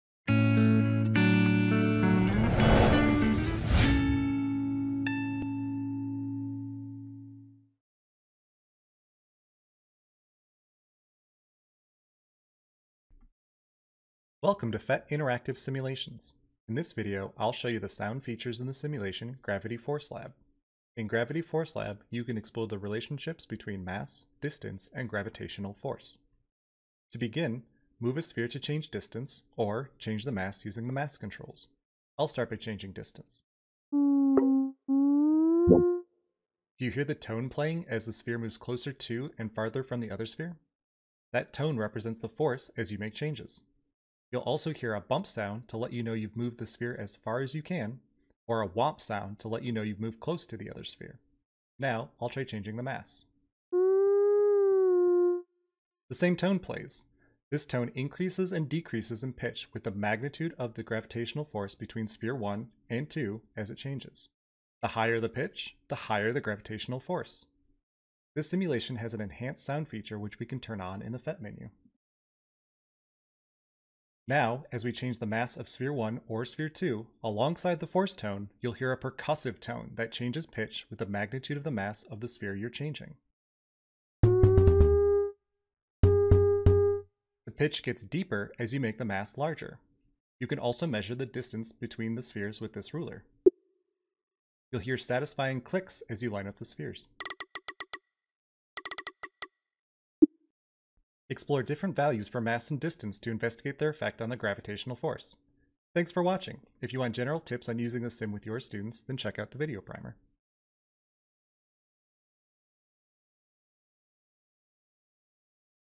Sound Features - Gravity Force Lab
This video describes the sound features in Gravity Force Lab. Check out the For Teachers section of the Gravity Force Lab simulation page on our website for the video primer, tips, and teacher-submitted activities!